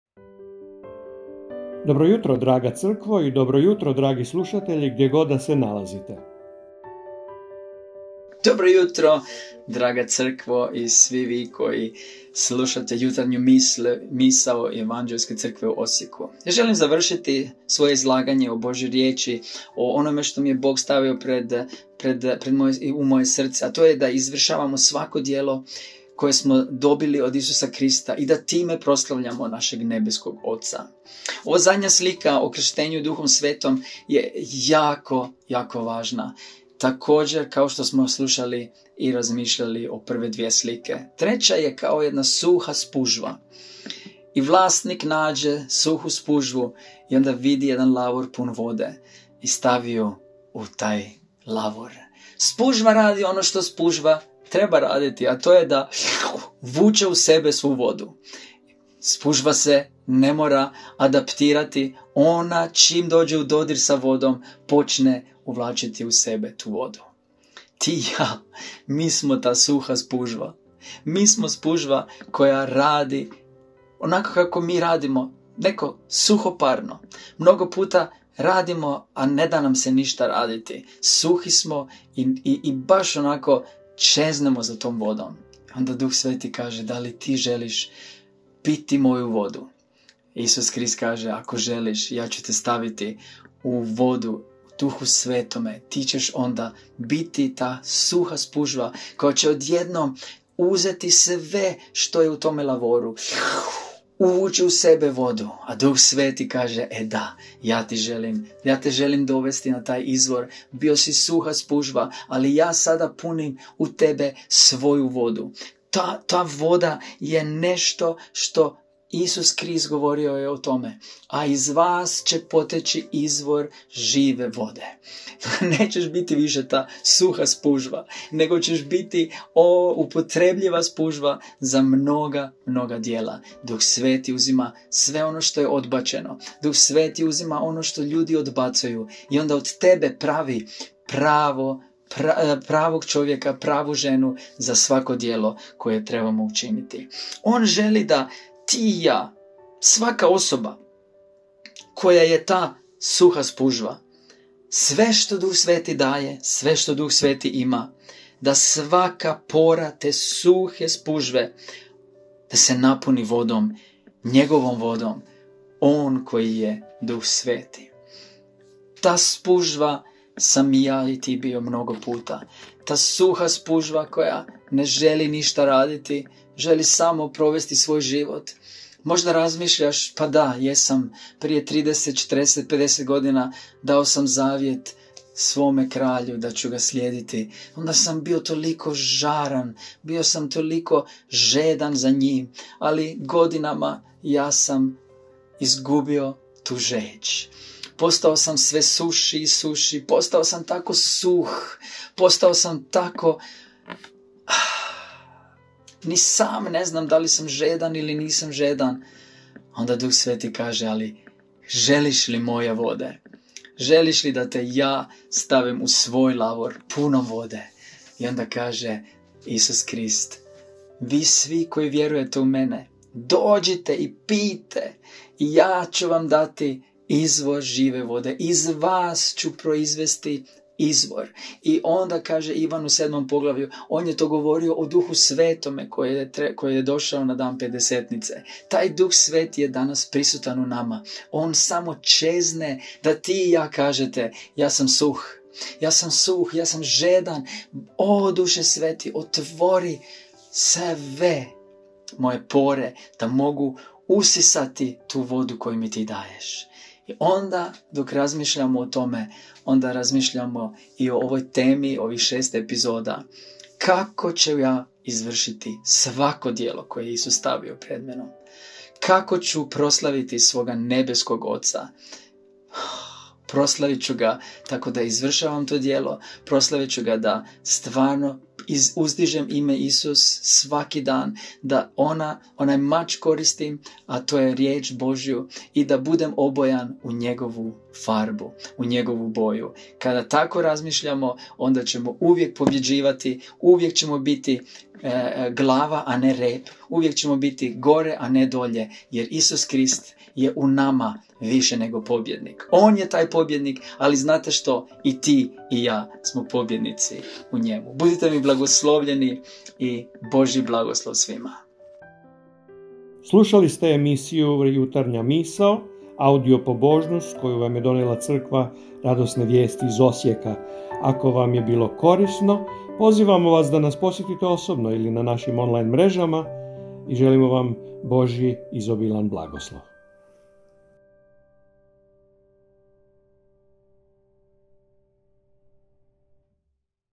Jutarnja misao | 6. dio - Izvršiti svako djelo i proslaviti nebeskog Oca | subota - 14.09.2024
Govornik - Zoran Kovačević
Grafika i mediji - Denisa Dželajlija
Autorska prava - EPC Osijek
Pastor - Damir Špoljarić